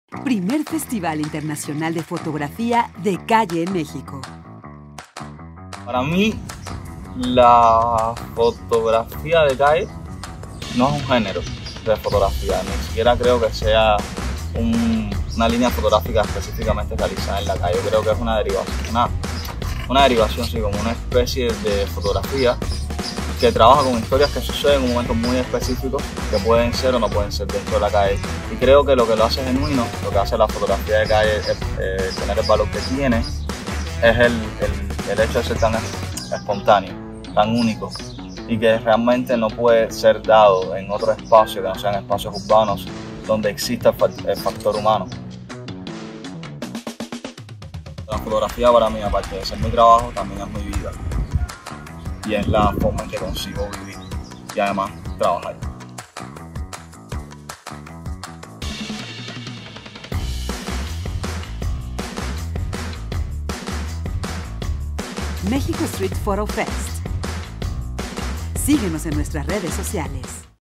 MXSPF presenta: Víctor Cervantes
Él es Víctor, fotógrafo cubano reconocido internacionalmente. Siempre ha creído que la fotografía es la vía más efectiva para compartir su mundo y las experiencias que tiene durante el tiempo que lo habite.

Podrás ver su trabajo en la exposición MXSPF 2023:

- Inauguración: 28 de octubre 2023.
- Lugar: Centro Educativo y Cultural Manuel Gómez Morin. Querétaro, México.